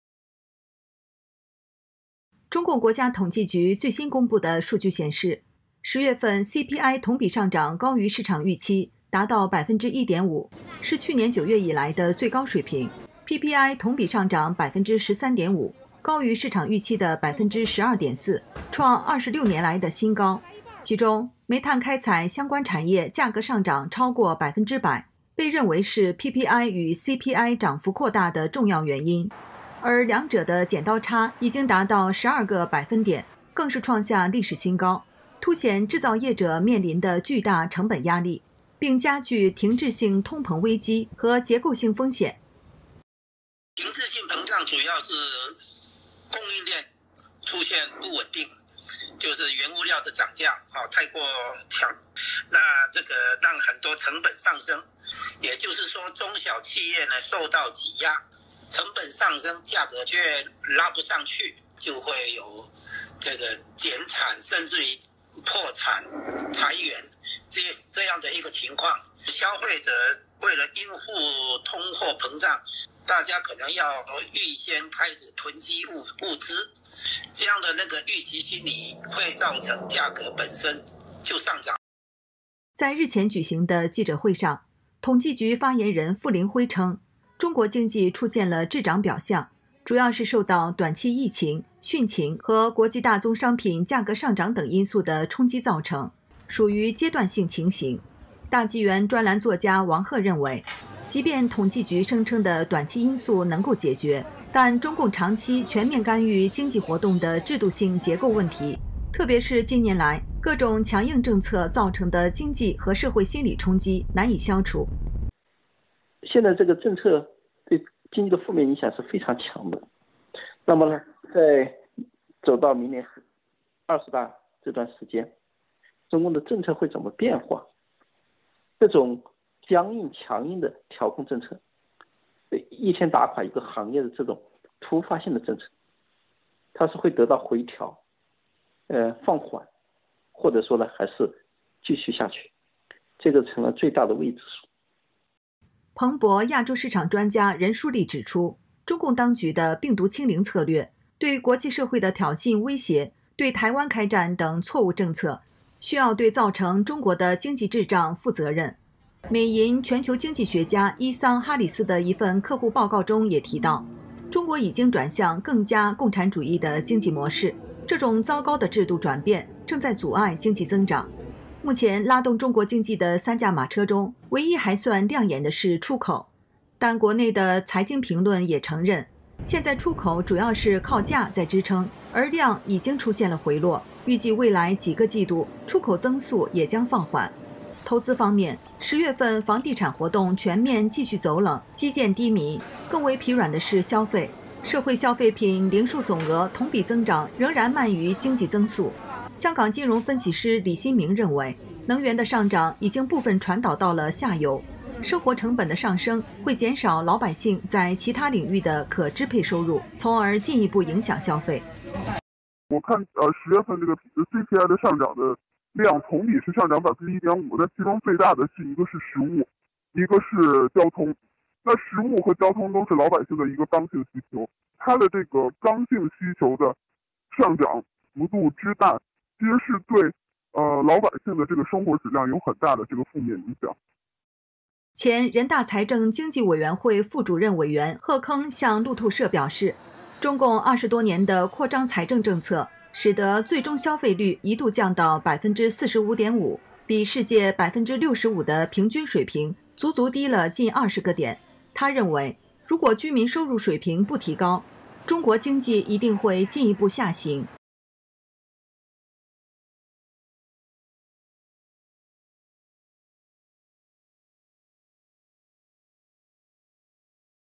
政策傷害經濟 中國滯脹危機凸顯｜@ChinaBreakingNewsTV ｜20211123
中共國家統計局最新公布的數據顯示，10月份CPI同比上漲高於市場預期，達到1.5%，是去年9月以來的最高水平；PPI同比上漲13.5%，高於市場預期的12.4%，創26年來的新高。

其中，煤炭開採相關產業價格上漲超過100%，被認為是PPI與CPI漲幅擴大的重要原因。

而兩者的剪刀差已經達到12個百分點，更是創下歷史新高。凸顯製造業者面臨的巨大成本壓力，並加劇停滯性通膨危機和結構性風險。

台灣宏觀經濟學家吳嘉隆：「停滯性膨脹主要是供應鏈出現不穩定，就是原物料的漲價太過強。那這個讓很多成本上升。也就是說，中小企業受到擠壓，成本上升價格卻拉不上去。就會有這個減產甚至於破產裁員這些這樣的一個情況。 消費者為了應付通貨膨脹, 大家可能要預先開始囤積物資，那個預期心理會造成價格本身就上漲。」

在日前舉行的記者會上，統計局發言人付凌暉稱，中國經濟出現了滯漲「表象」，主要是受到短期疫情、汛情和國際大宗商品價格上漲等因素的衝擊造成，屬於階段性情形。

大紀元專欄作家王赫認為，即便統計局聲稱的短期因素能夠解決，但中共長期全面干預經濟活動的制度性結構問題，特別是今年來各種強硬政策造成的經濟和社會心理衝擊，難以消除。

大紀元專欄作家王赫：「現在這個政策對經濟的負面影響是非常強的。那麼走到明年二十大這段時間，中共的政策會怎麼變化？這種僵硬強硬的調控政策，一天打垮一個行業的這種突發性的政策，它是會得到回調、放緩或者說呢還是繼續下去，這個成了最大的未知數。」

彭博亞洲市場專家任淑莉（Shuli Ren）指出，中共當局的「病毒清零」策略、對國際社會的挑釁、威脅對台灣開戰等錯誤政策，需要對造成中國的經濟滯脹負責任。

美銀全球經濟學家伊桑·哈里斯（Ethan Harris）的一份客戶報告中也提到，中國已經轉向更加「共產主義」的經濟模式，這種糟糕的制度轉變正在阻礙經濟增長。

目前，拉動中國經濟的三駕馬車中，唯一還算亮眼的是出口。但國內的財經評論也承認，現在的出口主要是靠「價」在支撐，而「量」已經出現了回落，預計未來幾個季度出口增速也將放緩。

投資方面，10月份房地產活動全面繼續走冷，基建低迷；更為疲軟的是消費，社會消費品零售總額同比增長仍然慢於經濟增速。

香港金融分析師李心明認為，能源的上漲已經部份傳導到了下游，生活成本的上升會減少老百姓在其它領域的可支配收入，從而進一步影響消費。

香港金融分析師李心明：「我看10月份這個CPI 的上漲的量同比是上漲1.5%。但是其中最大的一個是食物，一個是交通，那食物和交通都是老百姓的一個剛性需求。那他的這個剛性需求的上漲幅度之大，其實是對老百姓的這個生活質量有很大的負面影響。」

前人大財政經濟委員會副主任委員賀鏗向路透社表示，中共20多年的「擴張財政」政策，使得最終消費率一度降到45.5%，比世界65%的平均水平，足足低了近20個點。

他認為，如果居民收入水平不提高，中國經濟一定會進一步下行。